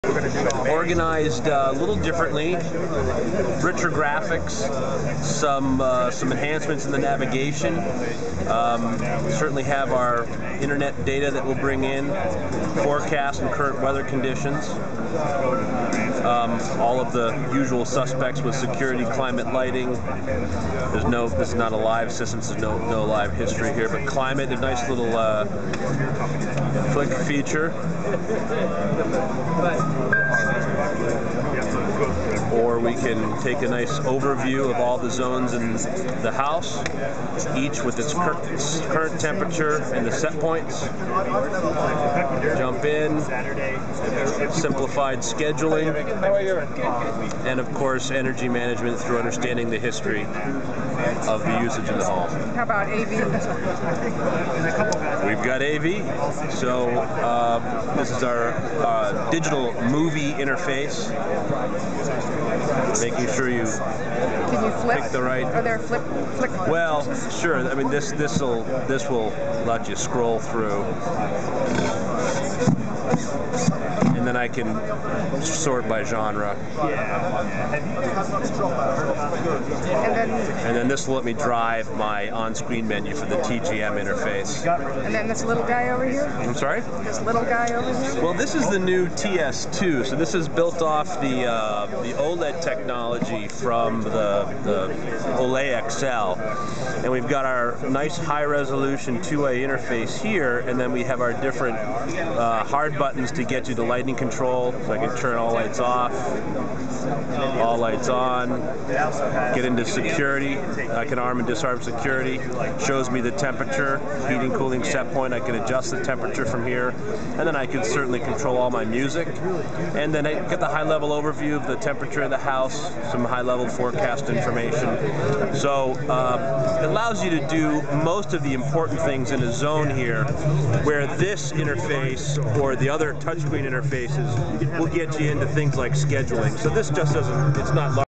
Elan Home Systems demos g! home automation system at ISE 2010
Elan's worldwide debut of the g! home automation system took place in Amsterdam at Integrated Systems Europe (ISE). The product finally brings complete integration between Elan and sister company HomeLogic. Joe Lautner of Homelogic/Elan flicks his way through thermostats, lighting controls, audio, video, security and more.